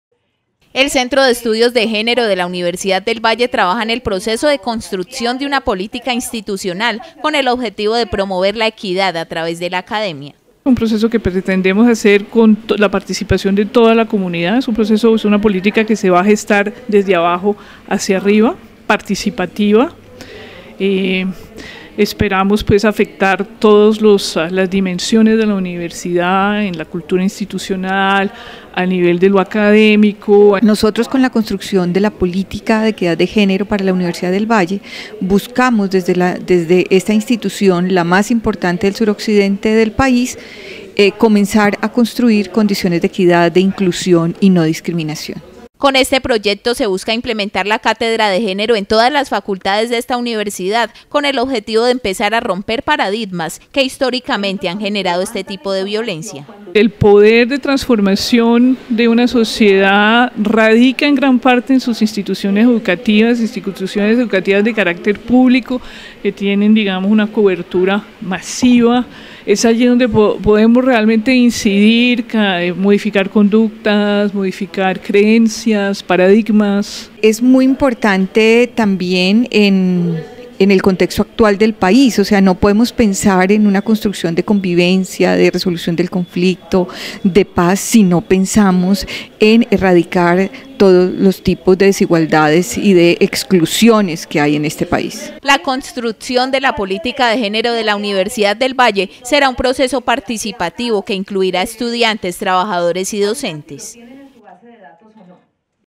La Universidad del Valle implementa programa para fomentar el respeto a las mujeres
El Centro de Estudios de Género de la Universidad del Valle trabaja en el proceso de construcción de una política institucional con el objetivo de promover la equidad a través de la academia.

Con este proyecto se busca implementar la cátedra de género en todas las facultades de esta universidad con el objetivo de empezar a romper paradigmas que históricamente han generado este tipo de violencia.

La construcción de la política de género de la Universidad del Valle será un proceso participativo que incluirá a estudiantes, trabajadores y docentes.